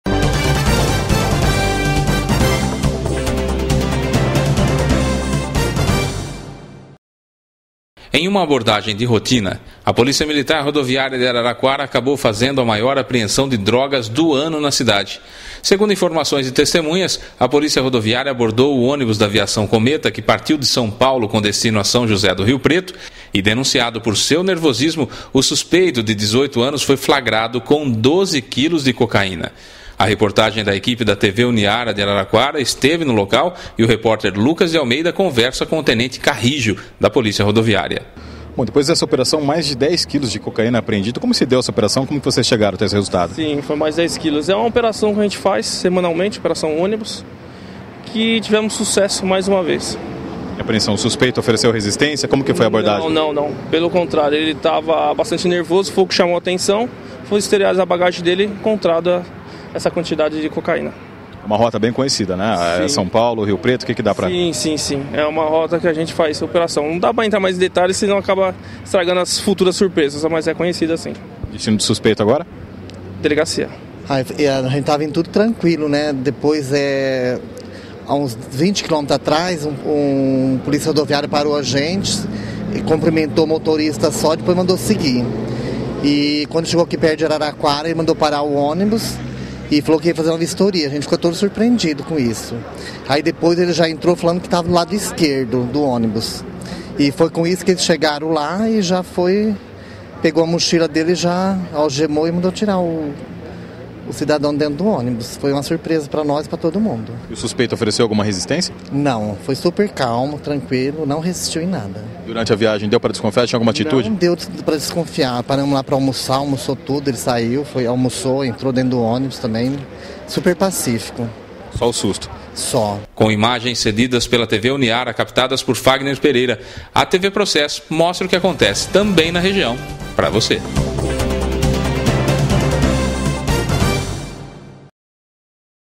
Maior apreensão de cocaína em Araraquara
Esta foi a maior apreensão do ano deste tipo de entorpecente na cidade vizinha. O rapaz que levava a droga tem apenas 18 anos.

A Polícia Rodoviária apreendeu 12 kg de cocaína, divididos em tijolos, com o passageiro de um ônibus durante uma blitz de rotina realizada em frente à base da corporação, na rodovia Washington Luís (SP-310).

Segundo o tenente Nelson Carrijo, da Polícia Rodoviária, operações como esta são feitas semanalmente. Aleatoriamente, ônibus intermunicipais de passageiros são revistados.

"Revistamos a bolsa de alguns passageiros e, como o suspeito estava muito nervoso, pedimos para ver a mochila, onde encontramos os tijolos de cocaína".

O desempregado Rodrigo B. S., de 18 anos, teria pego a droga em São Paulo e iria até São José do Rio Preto, cidade onde reside.

Rodrigo disse que era o dono da droga, porém, os policiais rodoviários acreditam que ele estivesse fazendo apenas o papel de "mula".

O caso será apresentado à Delegacia de Investigações sobre Entorpecentes (Dise) de Araraquara.

Segundo informações da Policia Civil de Araraquara, o suspeito deverá ser preso, acusado de tráfico de drogas.

Reportagem: Claudio Dias (Jornal Tribuna Impressa).

Lucas de Almeida (Tv Uniara)
Imagens: Fágner Pereira (TV Uniara)

Fotos: Daniel Barreto (Jornal Tribuna Impressa)

Narração e Edição: Fábio Pereira